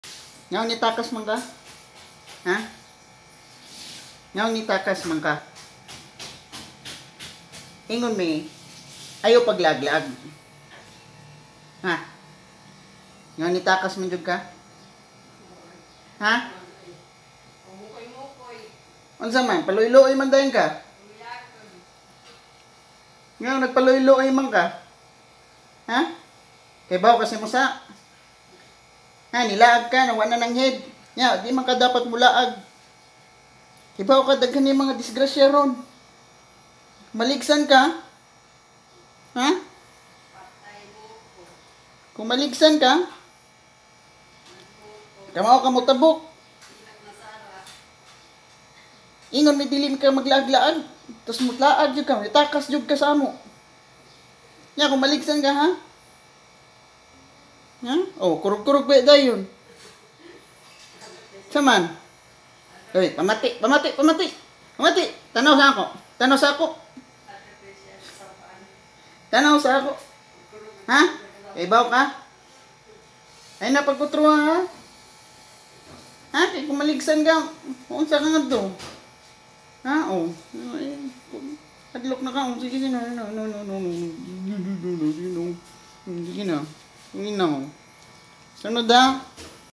Puppy eyes guilty dog (Mocco)
Mocco looks guilty after escaping